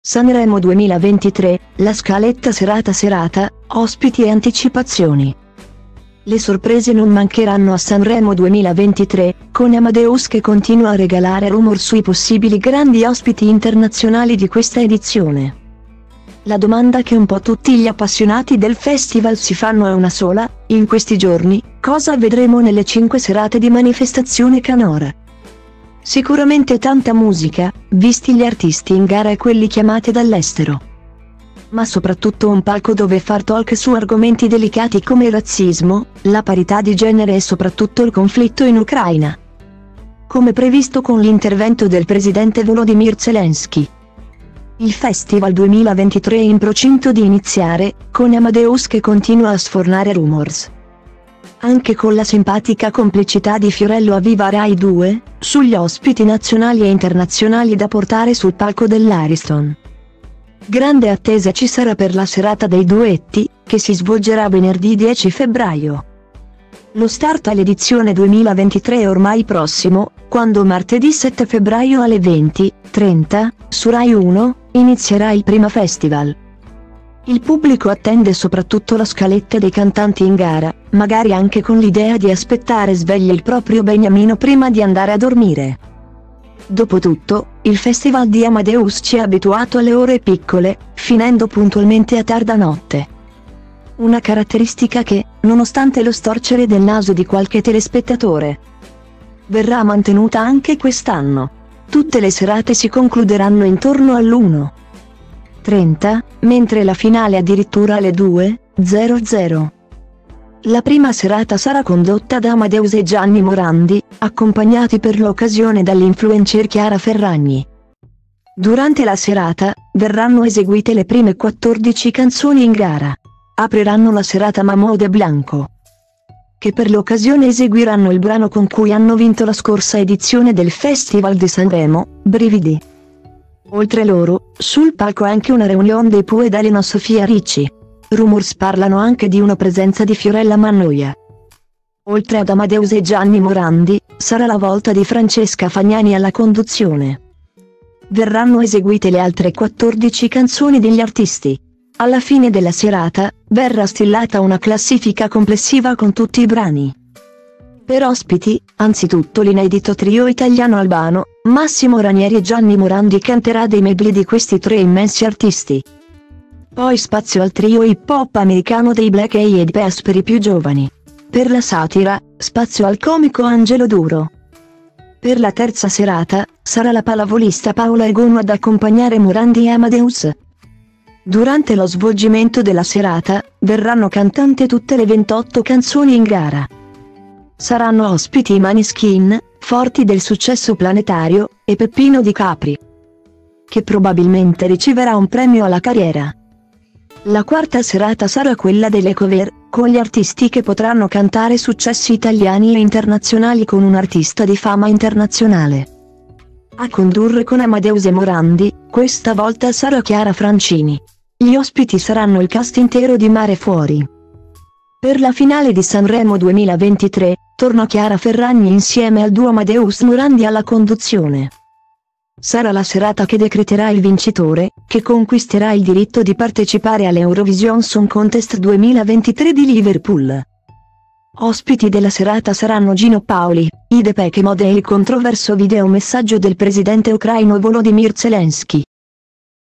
Sanremo 2023, la scaletta serata serata: ospiti e anticipazioni
Le sorprese non mancheranno a Sanremo 2023, con Amadeus che continua a regalare rumors sui possibili grandi ospiti internazionali di questa edizione.La domanda che un po’ tutti gli appassionati del Festival si fanno è una sola, in questi giorni: “Cosa vedremo nelle cinque serate di manifestazione canora?”.Sicuramente tanta musica, visti gli artisti in gara e quelli chiamati dall’estero,